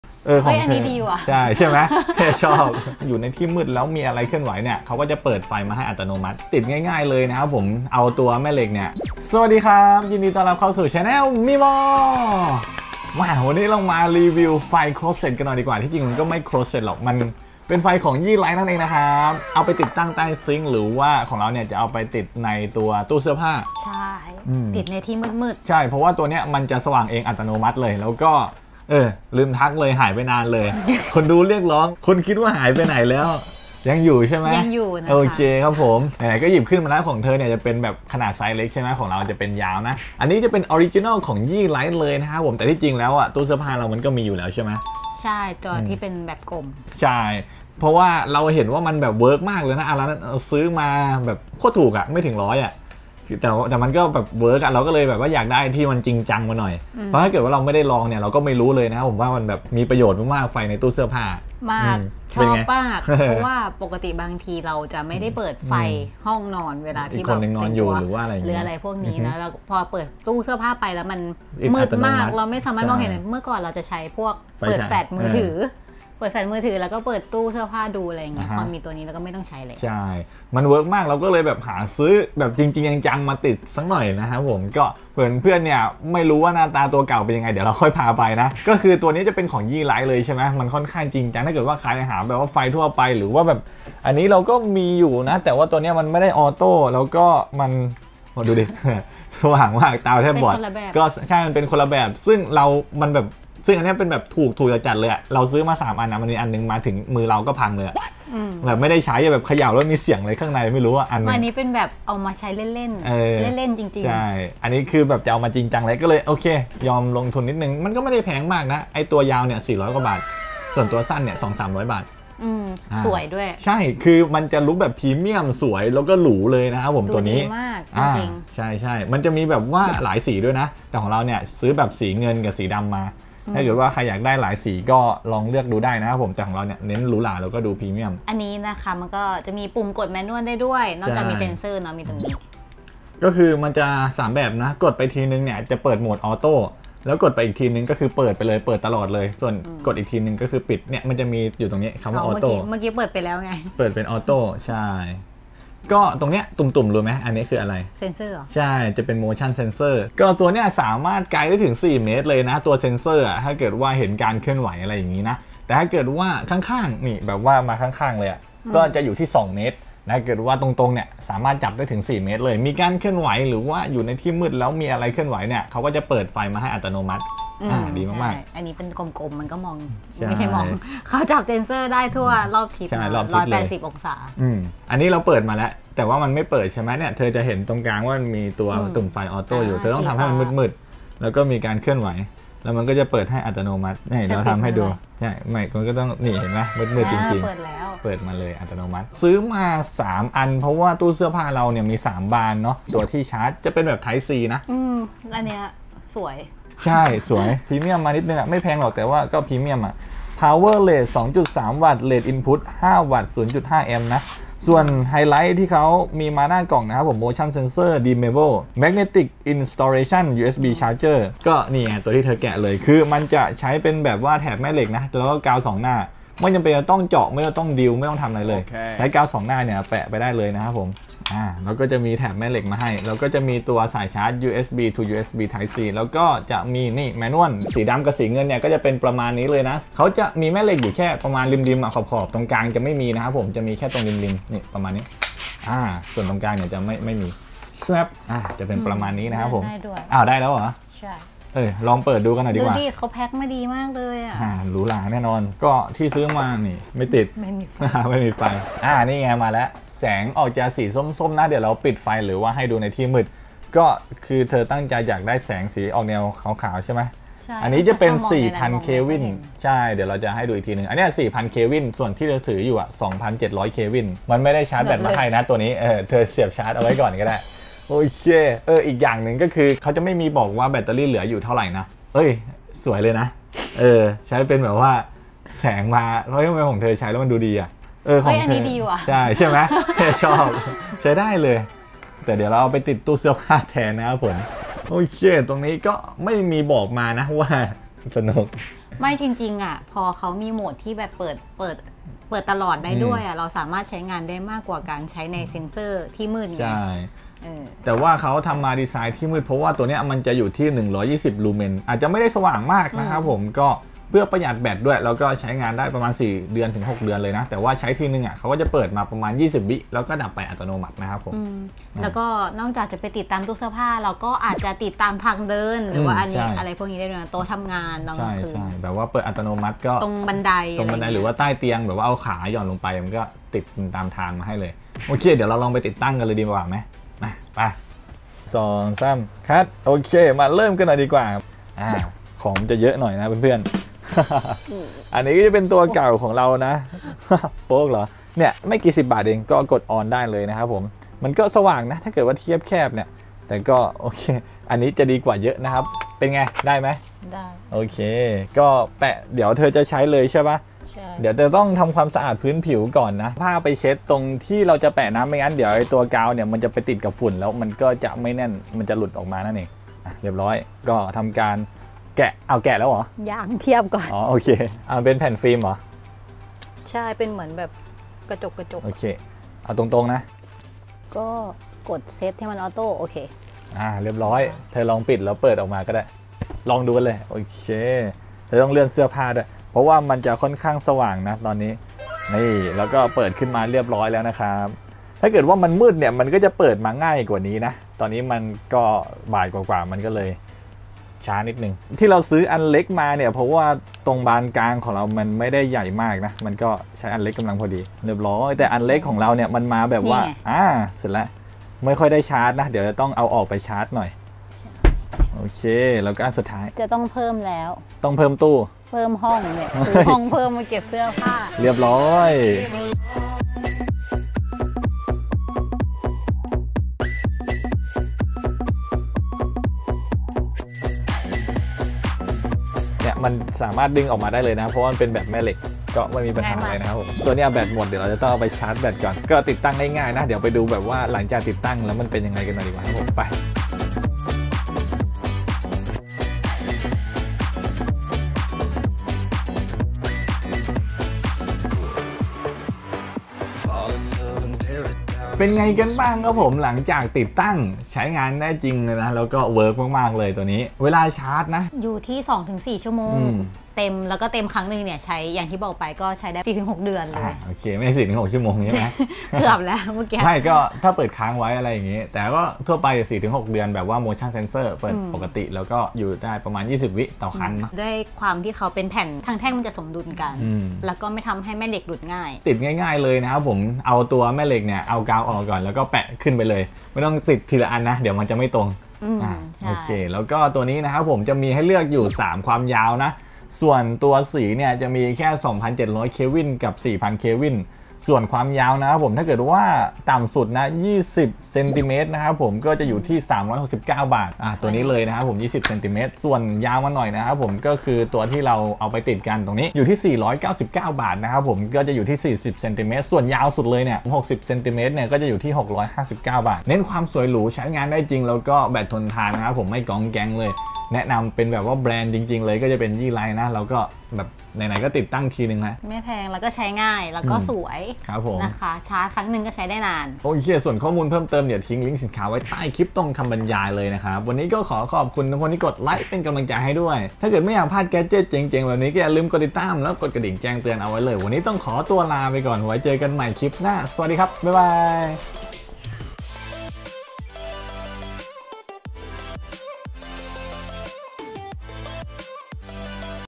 369 บาทแผ่นไฟเซ็นเซอร์ Yeelight ไม่ต้องเดินสายไฟ ติดตั้งง่ายด้วยแม่เหล็ก เปิด-ปิดอัตโนมัติ
ลิงค์สินค้า